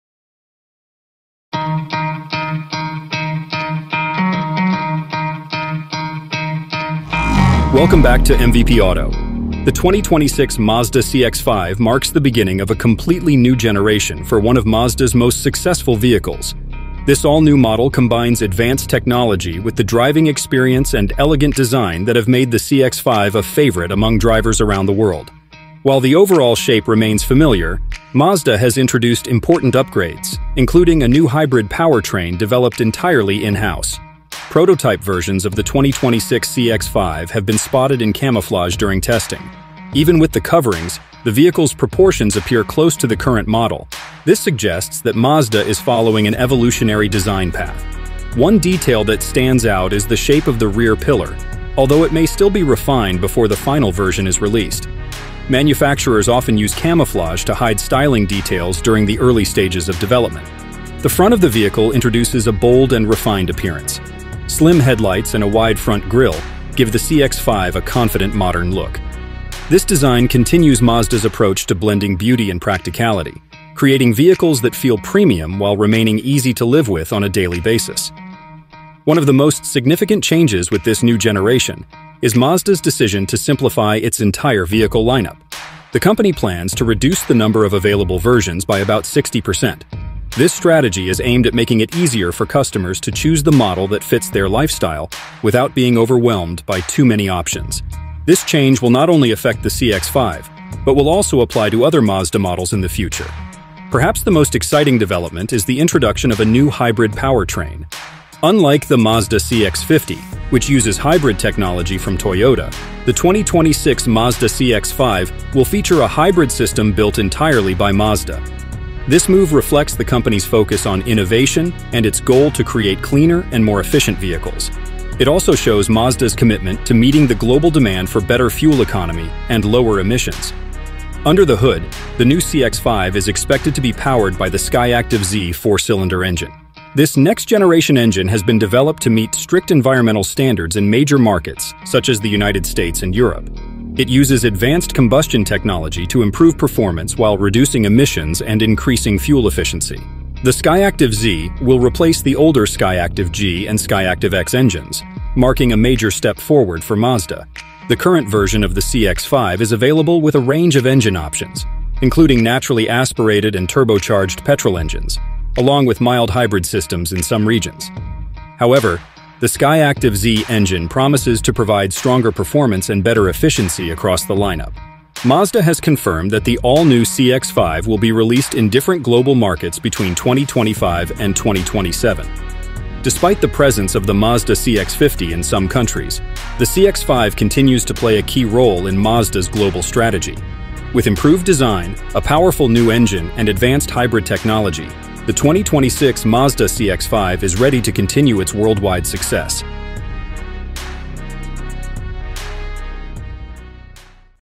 2026 Mazda CX-5 - Skyactiv-Z Engine and Mazda’s Hybrid Future!
2026 Mazda CX-5 - Skyactiv-Z Engine and Mazda’s Hybrid Future!

The 2026 Mazda CX-5 marks the beginning of a completely new generation for one of Mazda’s most successful vehicles. This all-new model combines advanced technology with the driving experience and elegant design that have made the CX-5 a favorite among drivers around the world. While the overall shape remains familiar, Mazda has introduced important upgrades, including a new hybrid powertrain developed entirely in-house.

Prototype versions of the 2026 CX-5 have been spotted in camouflage during testing. Even with the coverings, the vehicle’s proportions appear close to the current model. This suggests that Mazda is following an evolutionary design path. One detail that stands out is the shape of the rear pillar, although it may still be refined before the final version is released. Manufacturers often use camouflage to hide styling details during the early stages of development.

The front of the vehicle introduces a bold and refined appearance. Slim headlights and a wide front grille give the CX-5 a confident, modern look. This design continues Mazda’s approach to blending beauty and practicality, creating vehicles that feel premium while remaining easy to live with on a daily basis.

One of the most significant changes with this new generation is Mazda’s decision to simplify its entire vehicle lineup. The company plans to reduce the number of available versions by about sixty percent. This strategy is aimed at making it easier for customers to choose the model that fits their lifestyle without being overwhelmed by too many options. This change will not only affect the CX-5 but will also apply to other Mazda models in the future.

Perhaps the most exciting development is the introduction of a new hybrid powertrain. Unlike the Mazda CX-50, which uses hybrid technology from Toyota, the 2026 Mazda CX-5 will feature a hybrid system built entirely by Mazda. This move reflects the company’s focus on innovation and its goal to create cleaner and more efficient vehicles. It also shows Mazda’s commitment to meeting the global demand for better fuel economy and lower emissions.

Under the hood, the new CX-5 is expected to be powered by the Skyactiv-Z four-cylinder engine. This next-generation engine has been developed to meet strict environmental standards in major markets such as the United States and Europe. It uses advanced combustion technology to improve performance while reducing emissions and increasing fuel efficiency. The Skyactiv-Z will replace the older Skyactiv-G and Skyactiv-X engines, marking a major step forward for Mazda.

The current version of the CX-5 is available with a range of engine options, including naturally aspirated and turbocharged petrol engines, along with mild-hybrid systems in some regions. However, the Skyactiv-Z engine promises to provide stronger performance and better efficiency across the lineup.

Mazda has confirmed that the all-new CX-5 will be released in different global markets between 2025 and 2027. Despite the presence of the Mazda CX-50 in some countries, the CX-5 continues to play a key role in Mazda’s global strategy. With improved design, a powerful new engine, and advanced hybrid technology, the 2026 Mazda CX-5 is ready to continue its worldwide success.